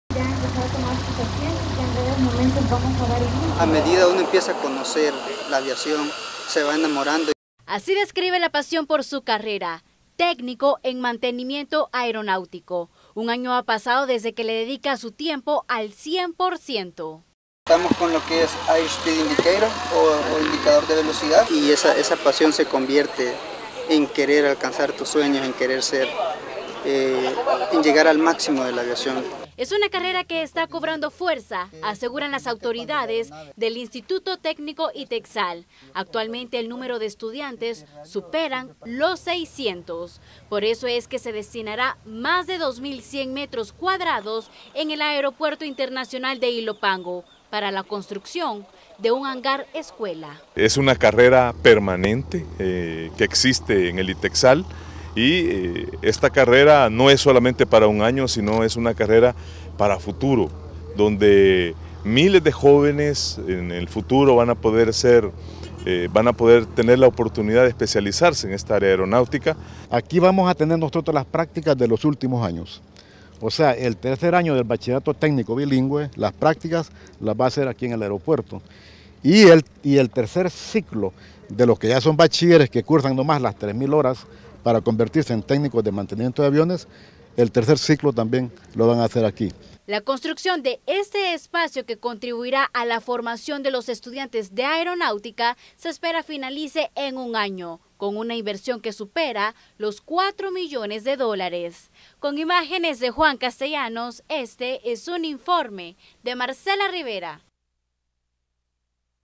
CN: Aliansa CEPA - ITEXAL
La Comisión Ejecutiva Portuaria autónoma y el Instituto Técnico de Ex Alumnos Salesianos, firmaron un convenio que beneficiara a los estudiantes de aeronáutica.